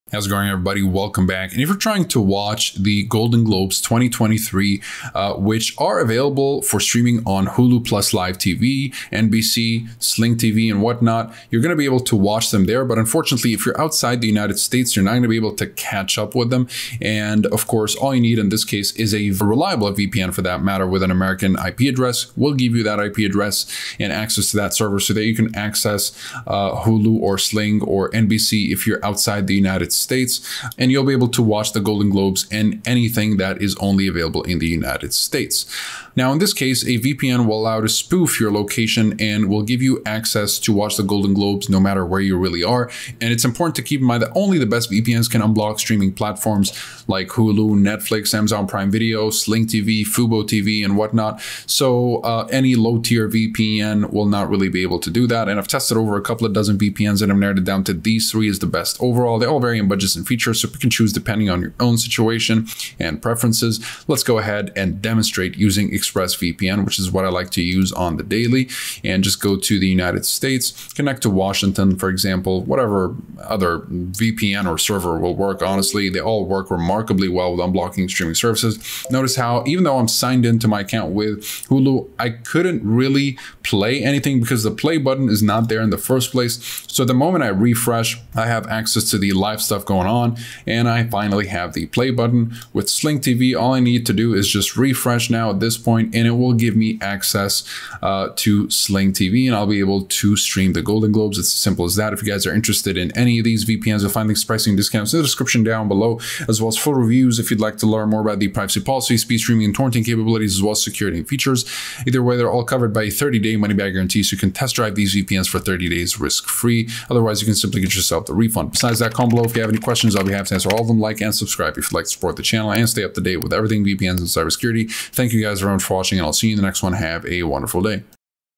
How to Watch the 2025 Golden Globes Live From Anywhere
How to Watch the 2025 Golden Globes Live From Anywhere. Here are the best VPNs to unblock streaming services. These are also ideal for privacy, speed, torrenting & online security.

How's it going, everybody? Welcome back. And if you're trying to watch the Golden Globes 2025, which are available for streaming on Hulu, plus live TV, NBC Sling TV and whatnot, you're going to be able to watch them there. But unfortunately, if you're outside the United States, you're not gonna be able to catch up with them. And of course, all you need in this case is a reliable a VPN for that matter with an American IP address will give you that IP address and access to that server so that you can access Hulu or sling or NBC if you're outside the United States. And you'll be able to watch the Golden Globes and anything that is only available in the United States. Now in this case, a VPN will allow to spoof your location and will give you access to watch the Golden Globes no matter where you really are. And it's important to keep in mind that only the best VPN is can unblock streaming platforms, like Hulu, Netflix, Amazon Prime, video, Sling TV, fubo, TV and whatnot. So any low tier VPN will not really be able to do that. And I've tested over a couple of dozen VPNs. And I've narrowed it down to these three is the best overall, they all vary in budgets and features. So you can choose depending on your own situation, and preferences. Let's go ahead and demonstrate using ExpressVPN, which is what I like to use on the daily and just go to the United States connect to Washington, for example, whatever other VPN or server will work honestly, they all work remarkably well with unblocking streaming services. Notice how even though I'm signed into my account with Hulu, I couldn't really play anything because the play button is not there in the first place. So the moment I refresh, I have access to the live stuff going on. And I finally have the play button. With Sling TV. All I need to do is just refresh now at this point and it will give me access to Sling TV and I'll be able to stream the Golden Globes. It's as simple as that if you guys are interested in any of these VPNs findings, pricing discounts the description down below, as well as for reviews if you'd like to learn more about the privacy policy, speech streaming and torrenting capabilities as well as security features. Either way, they're all covered by a 30 day money back guarantee. So you can test drive these EPS for 30 days risk free, otherwise you can simply get yourself the refund besides that combo if you have any questions I'll be happy to answer all of them like and subscribe if you'd like to support the channel and stay up to date with everything BB ends in cybersecurity. Thank you guys so much for watching. Have a wonderful day.

Hope you enjoyed my How to Watch the 2025 Golden Globes Live From Anywhere Video.